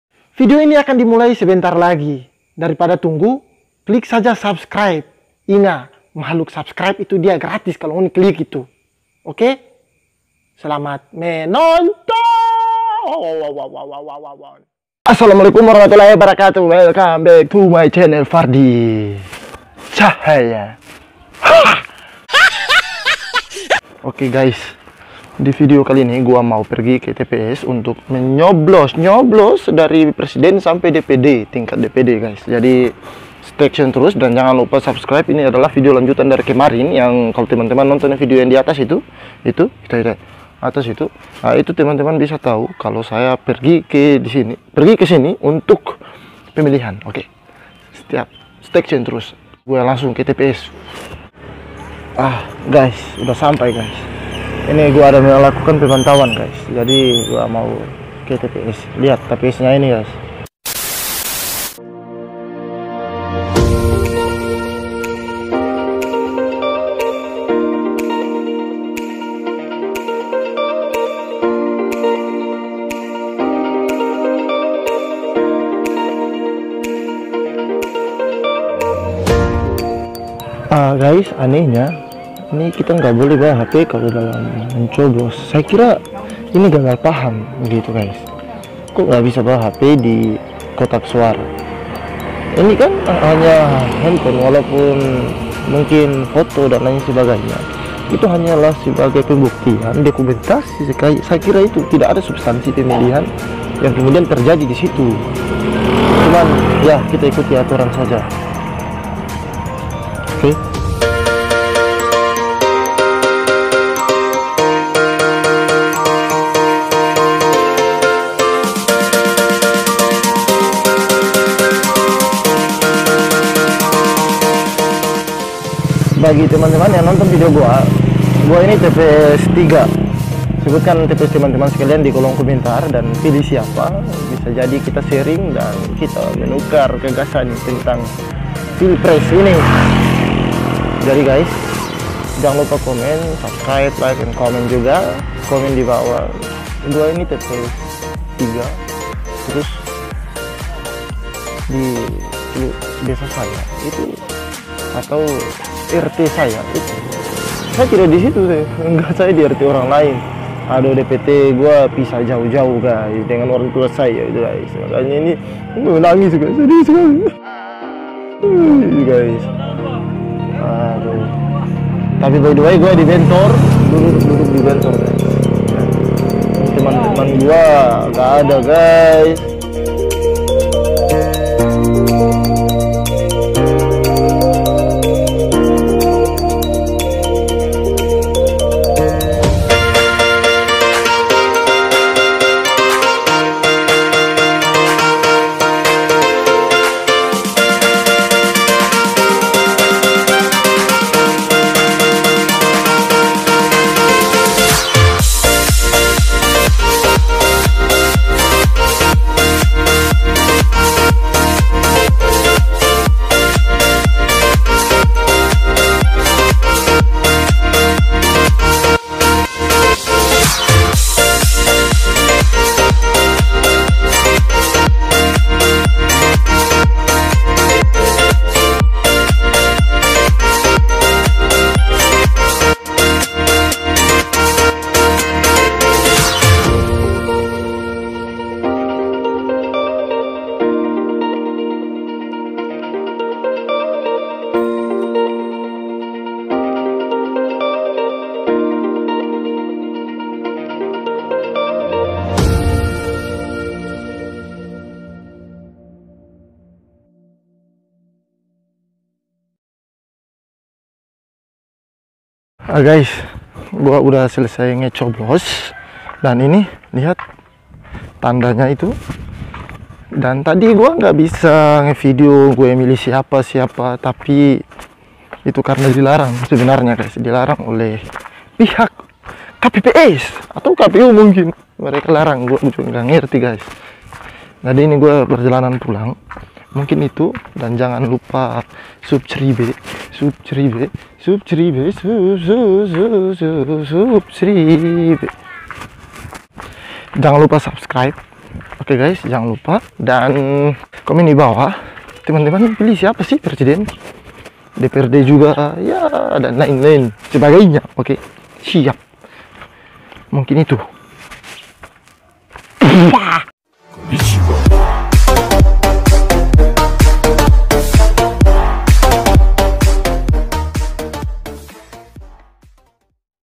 FCVlog suasana Pemilu 2019 (Maluku Utara)
Divideo kali ini saya pulang untuk mengikuti pemilihan dan membuat video tentang suasana Pemilu 2019 (Maluku Utara), pasti asyik. Jangan lupa nonton sampai habis ya. Selamat menonton.
Komentar
suasana Pemilu 2019 (Maluku Utara)